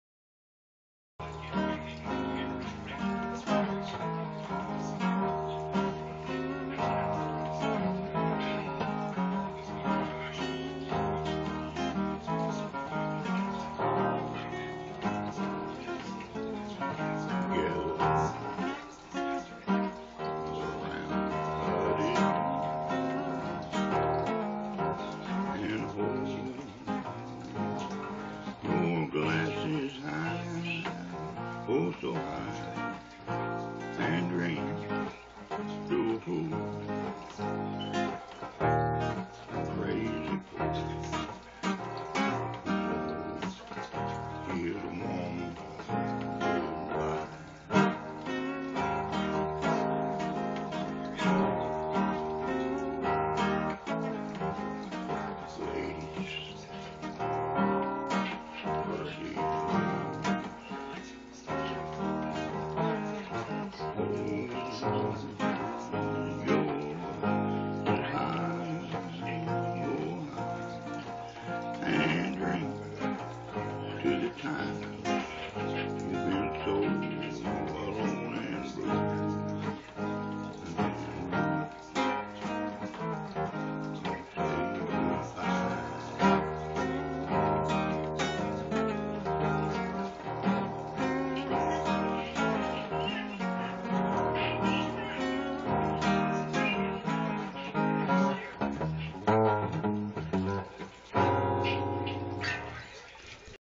Blues guitarist
Your never too old to play the blues...My dad wrote this song..